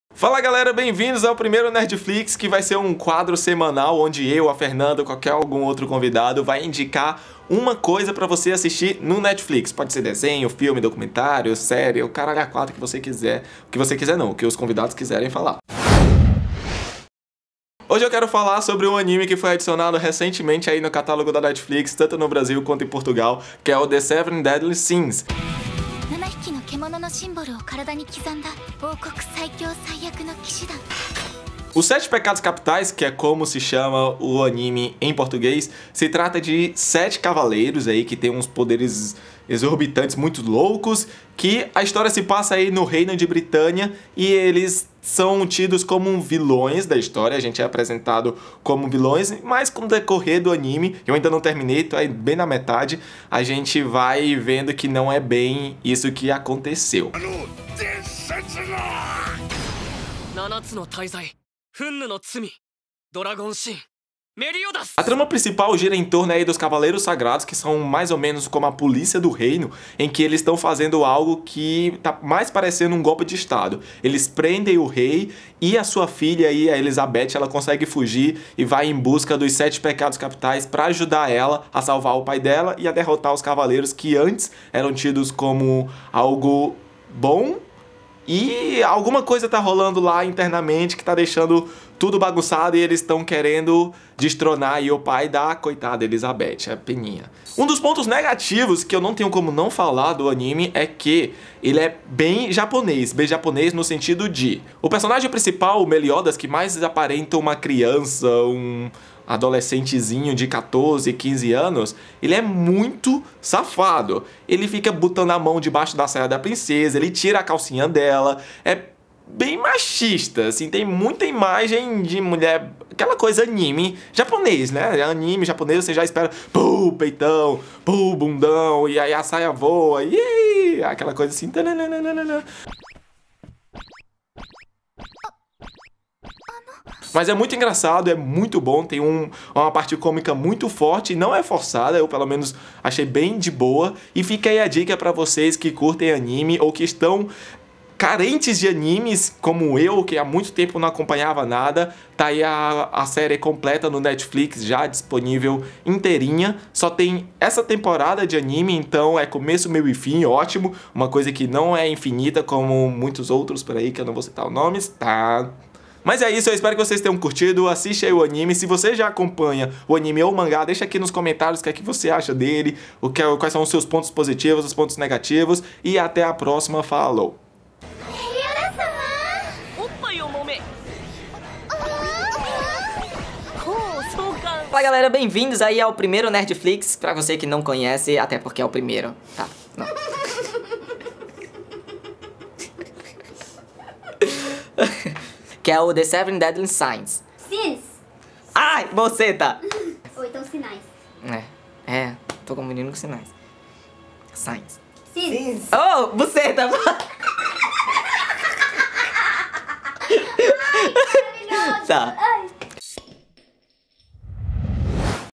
Anime The Seven Deadly Sins - NERDFLIX #01 | NETFLIX
Nerdflix é uma tag criada pelo geekbar para indicar séries, filmes ou documentários que estão disponíveis no Netflix Brasil!

A sugestão de hoje é o anime The Seven Deadly Sins!

Seja meu DIGIAMIGO também! Não esqueça de "CURTIR" o video e se "INSCREVER" aqui no canal!

SIGAM-ME OS BONS:

• Battletag Xaojo#1198